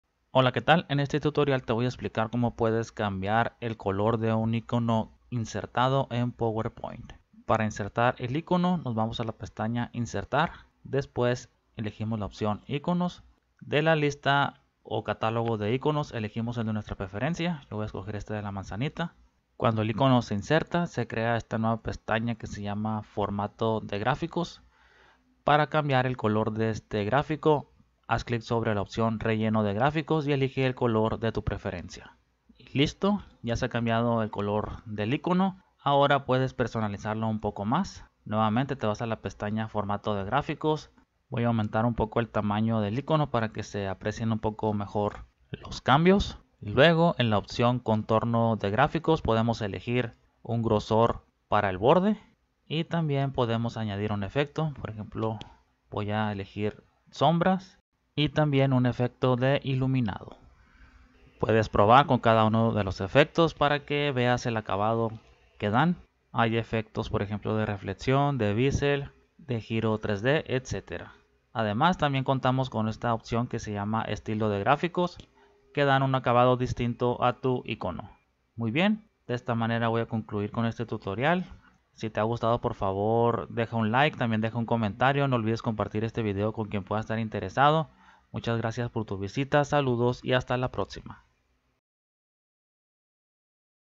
¿Cómo Cambiar El Color De Un Ícono Insertado En Power Point? | Dar Un Efecto y Cambiar Relleno
En este breve tutorial te explico como puedes cambiar el color de un ícono que insertes en una presentación o diapositiva de Power Point de Microsoft Office 365, además también puedes personalizar este ícono dando efectos y añadiendo un contorno.